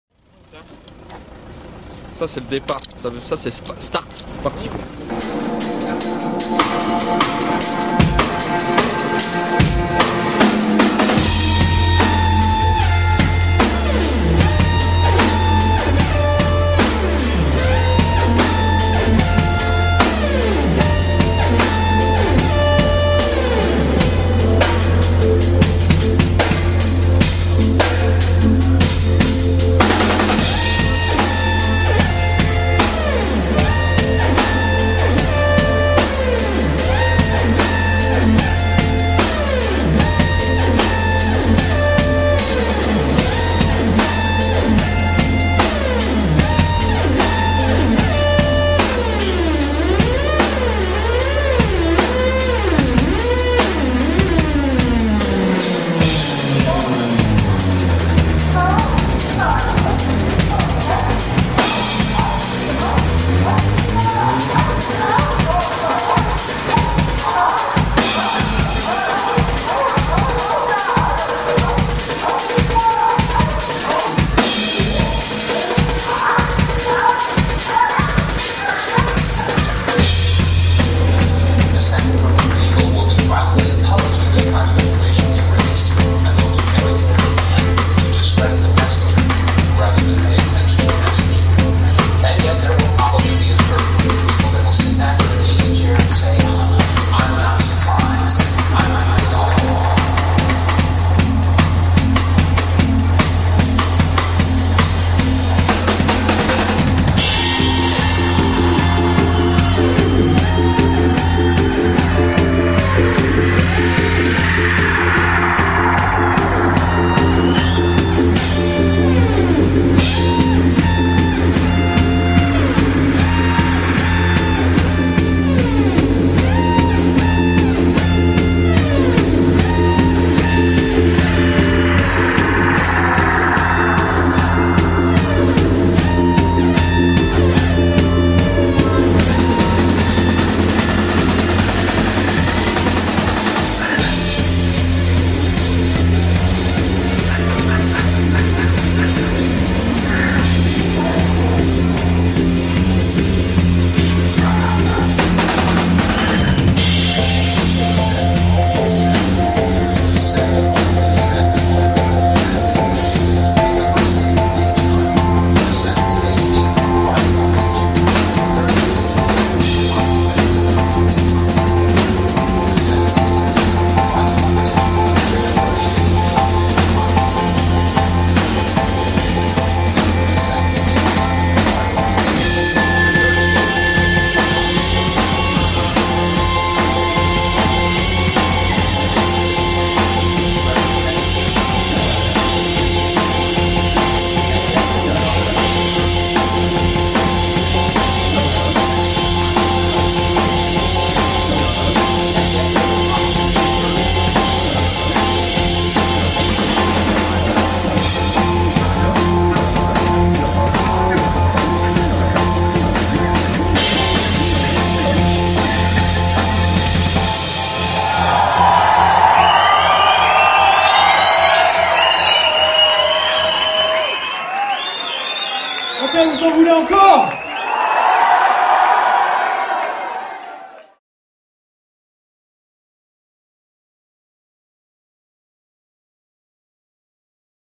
Teaser IPHAZE résidence de Février 2010
Teaser résidence salle du CAP (TOULOUSE)

Vidéo réalisé par Siham Ouattou (tournage et montage), Julien Escalier (régie vidéo), Fabien Lupy (conception 3D), Patrice Cadiou (habillage)